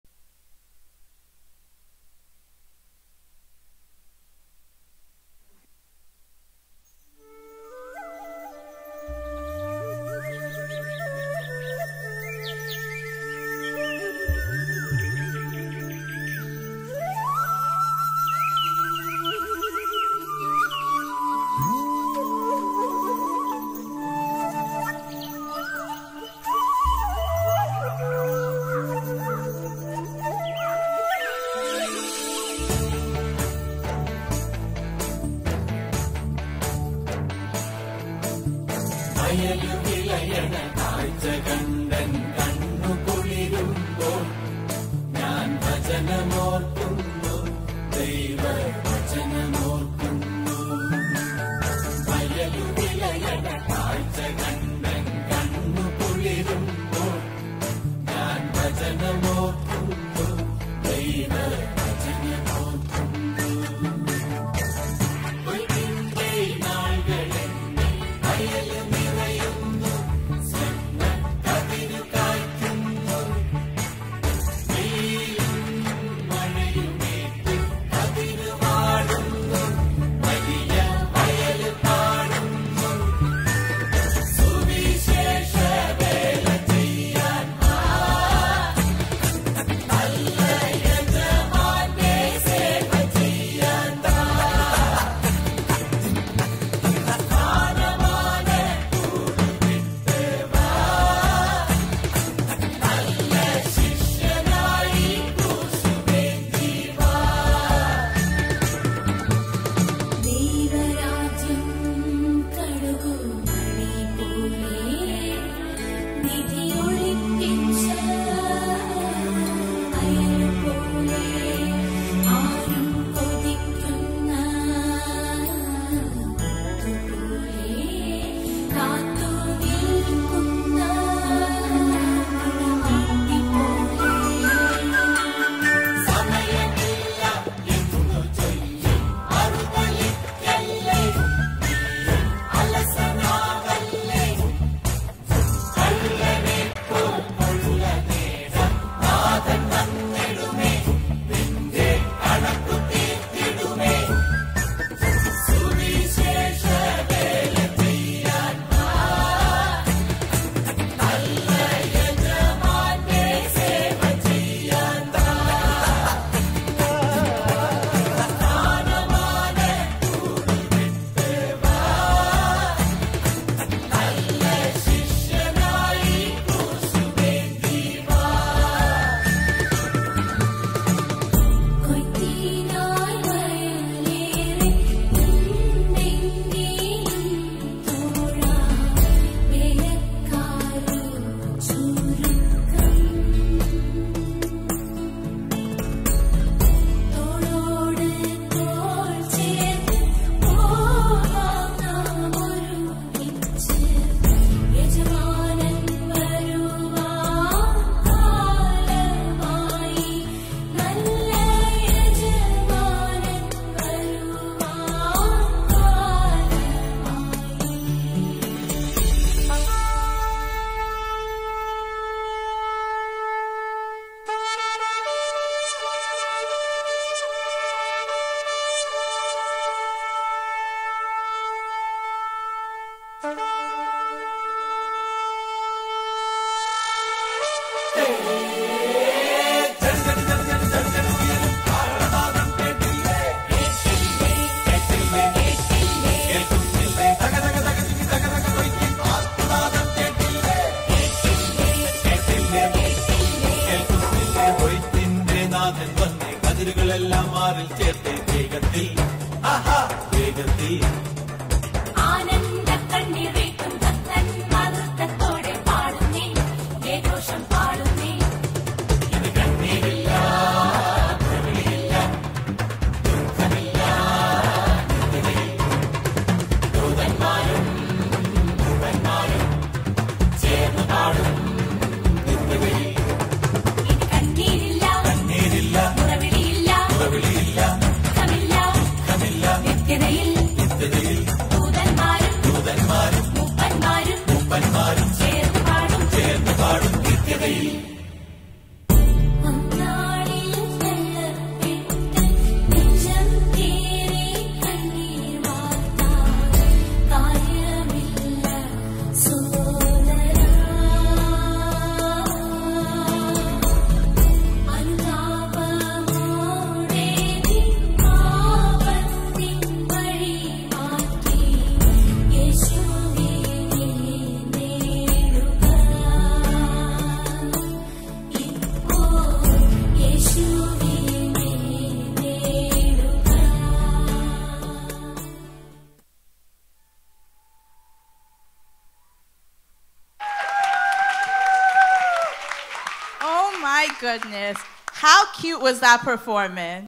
Action song Abigail & Aksah Sunday School Creative Night ICA NY 03/18/2023
Sunday School Creative Night ICA NY  03/18/2023

Download our FREE TV Application
UniTech TV HD Android APK